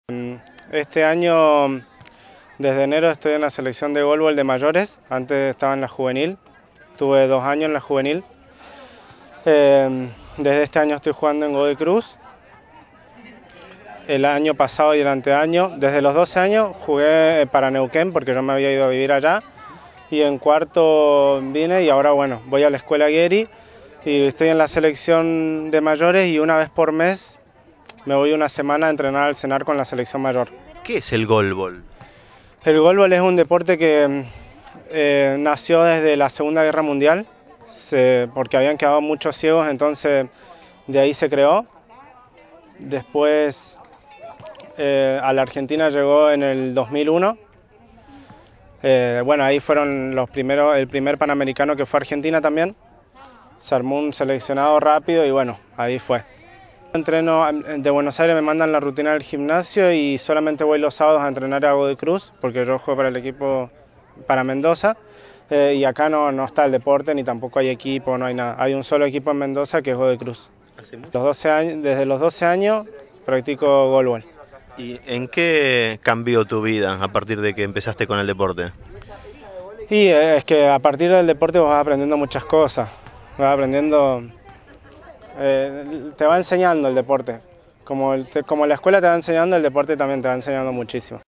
EL DEPORTE INTEGRA E INCLUYE
Sebastián Dominguez, discapacitado visual, señala la importancia del deporte en su formación y la participación que tiene a nivel provincial y nacional como jugador de Goalball